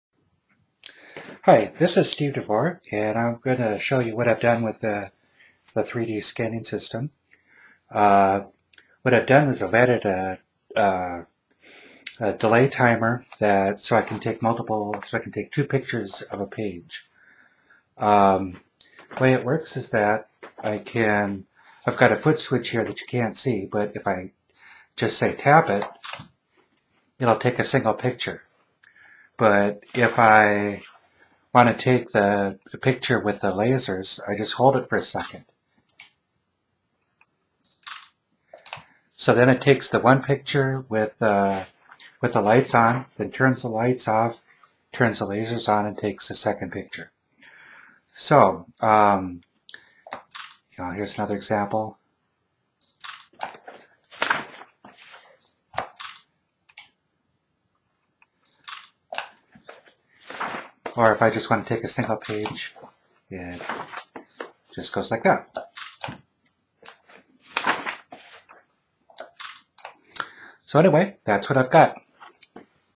DIY Book Scanner Laser-Based Scanning modification
This short example demonstrates how I modified the laser system.